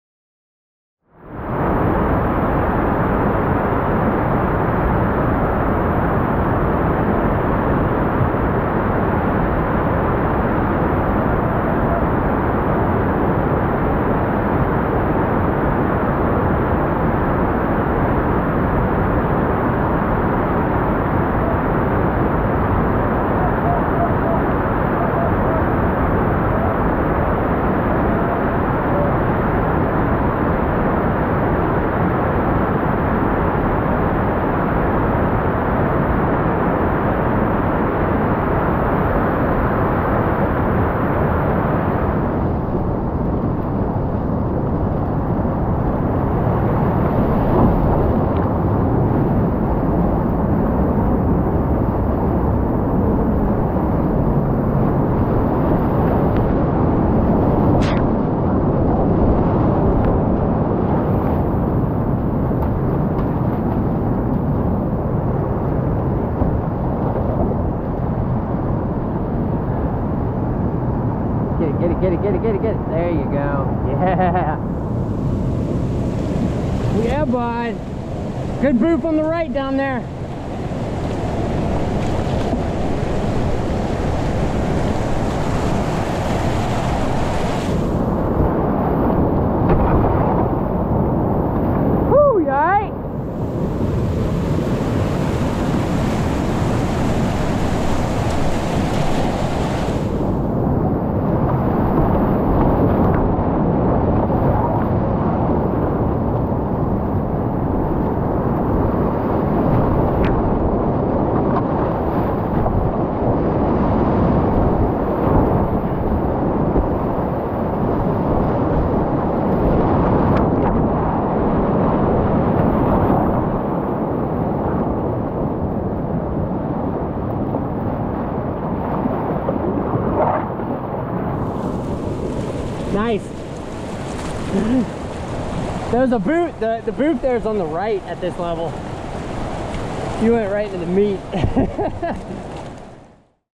Z Falls and 100 Yard Dash - Mildly Beatered
When Jared Seiler, Matt Fithian, and Evan Garcia hit the Upper B at a nice, friendly 300cfs, shenanigans are sure to ensue.  And when Floyd and I are still figuring out the lines of this whitewater maze, beatering is inevitable.  Here's the final quick clip of the Blackwater Media Week, a combo of the first two bigger rapids of the B, 100 Yard Dash and Z Falls, with shenanigans and beatering included.  Enjoy y'all!